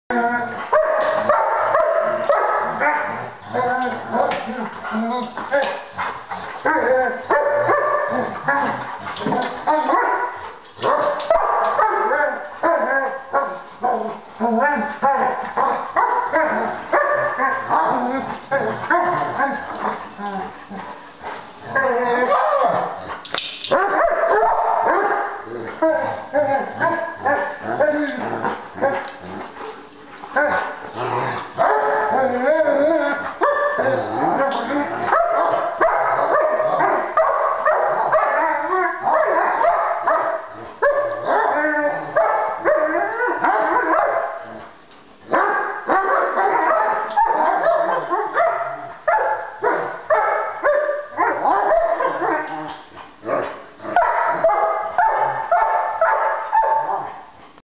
Giant Schnauzers Argue Over Dishwasher Rights
Arnie is growing up and at eight months old, he is getting bigger than Dudley. Here they are, just before dinner time, arguing over the dishwasher.